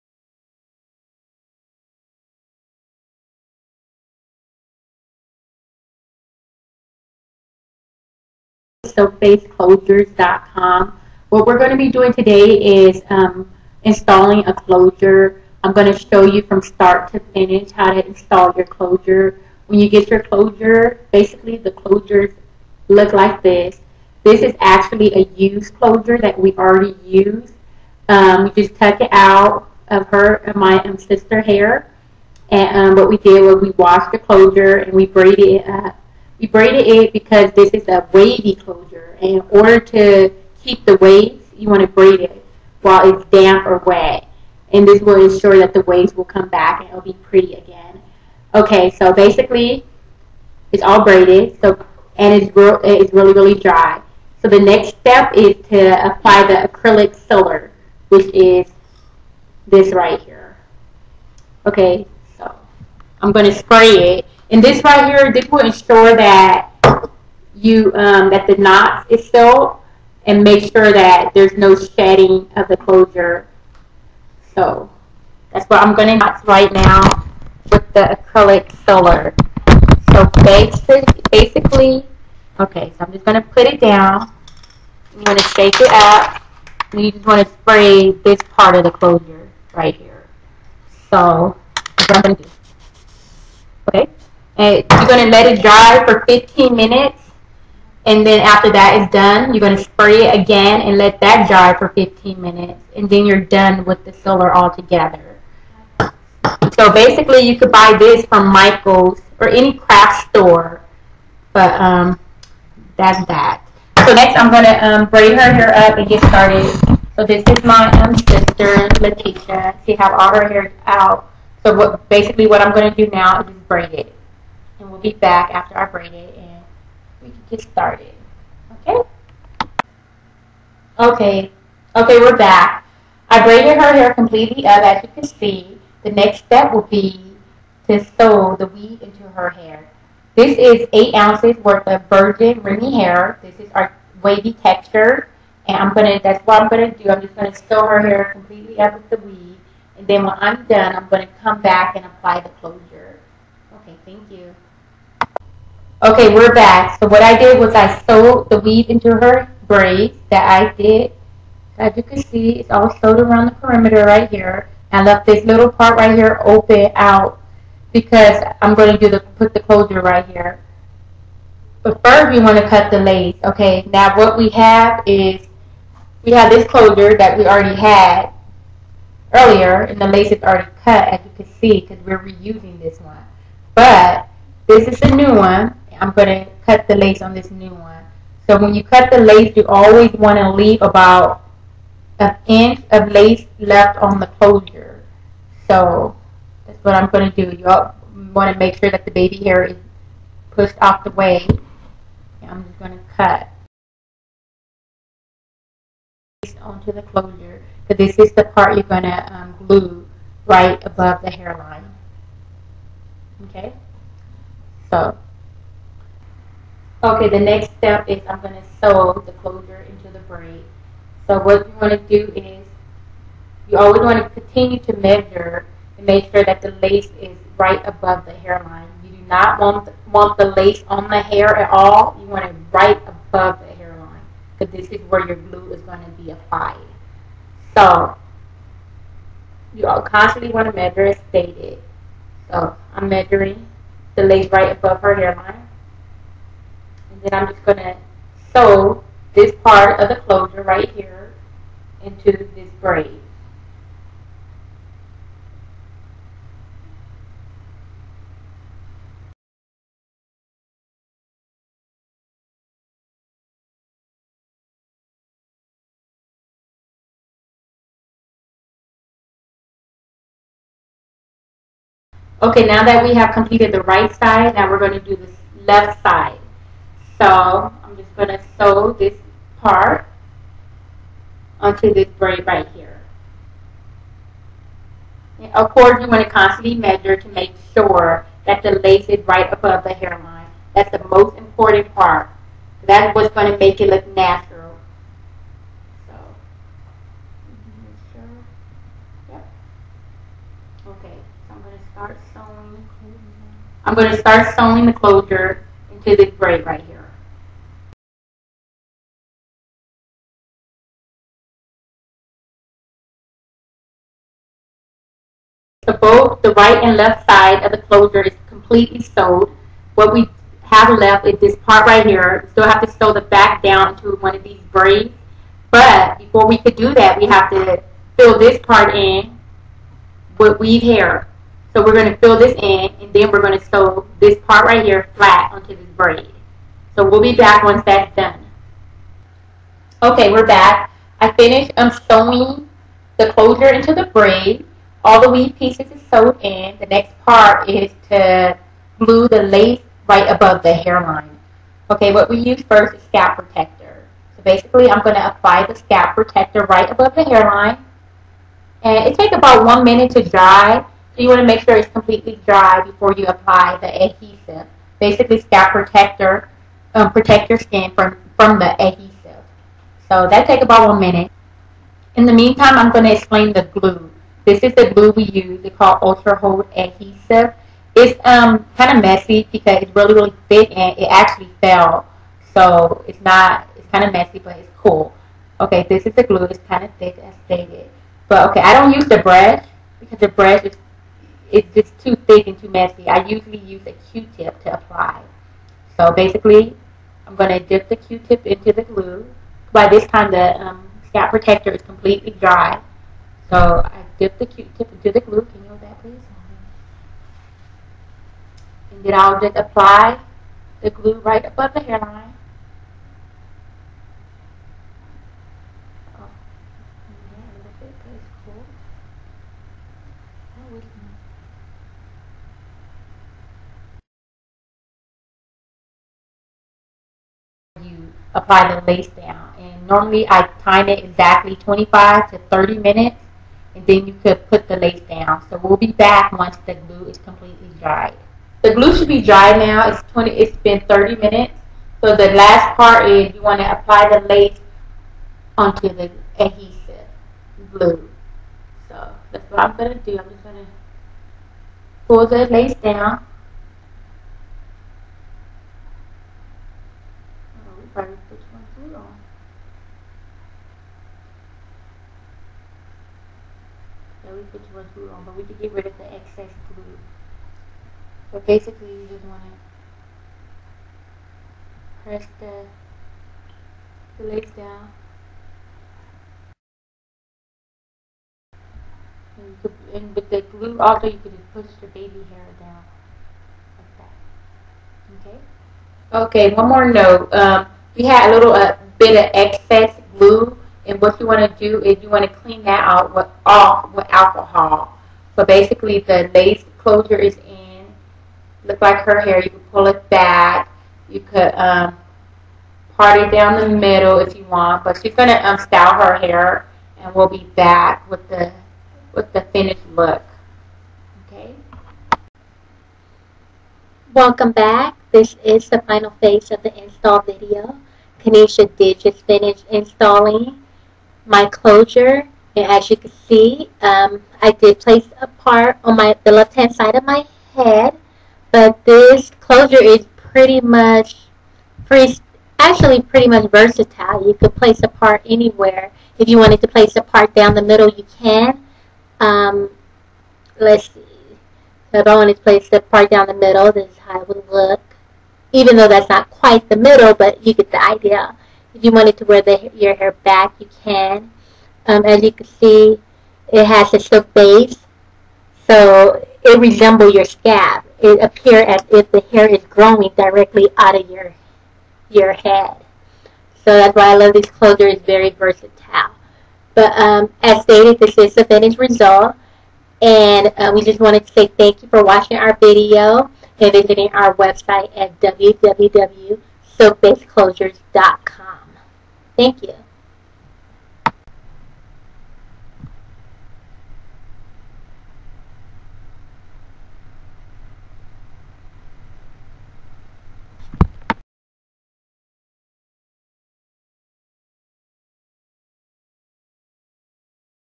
Install video for silk base closures
This video shows how to install a silk base closures. Courtesty of silkbaseclosures.com . This is an educational video on how to install a silk base closure.